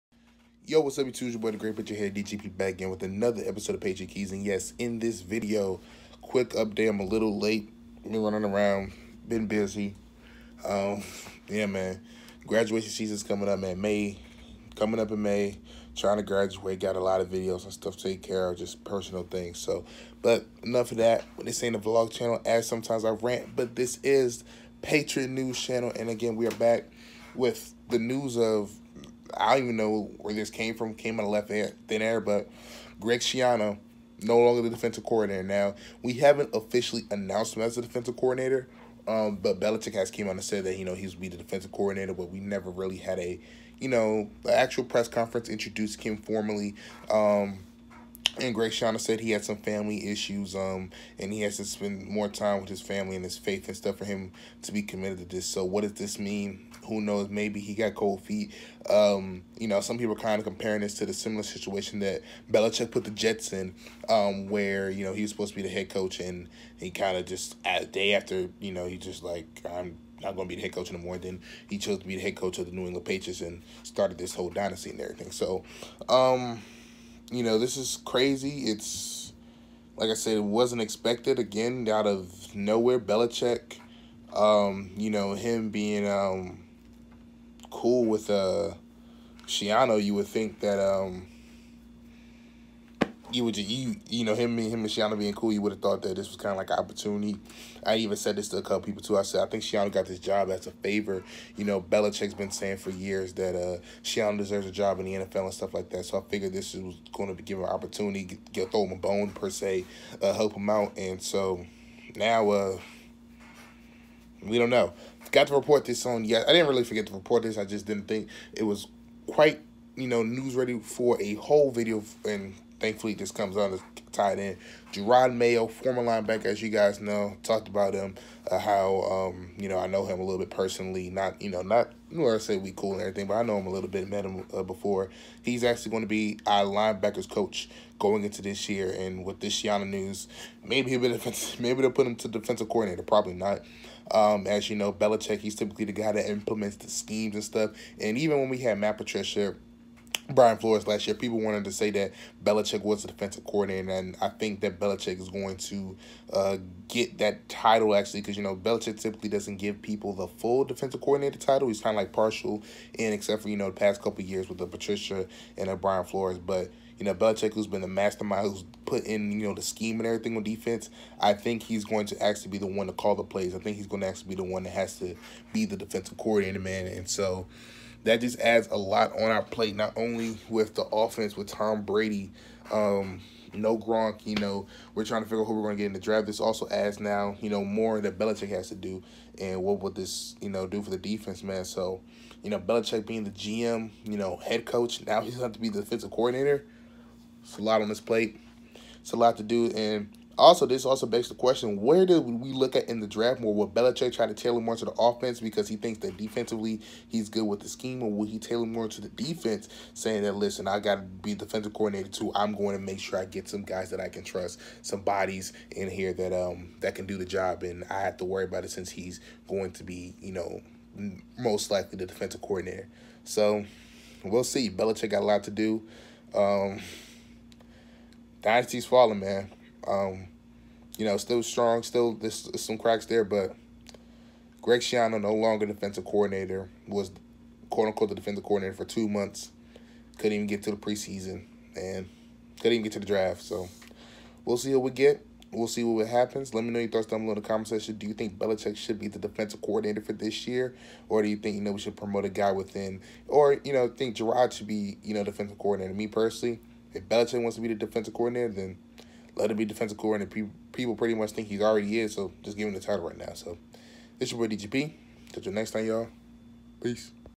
GREG SCHIANO STEPS DOWN | Jerod Mayo NEW LBs Coach | Patriots 🔑 to Victory
Schiano steps down from being our defensive coordinator for "family reasons". Also former Patriot LB Jerod Mayo is also our new LB coach. With Schiano gone where does this lead us for our DC? Does Belichick takeover as DC?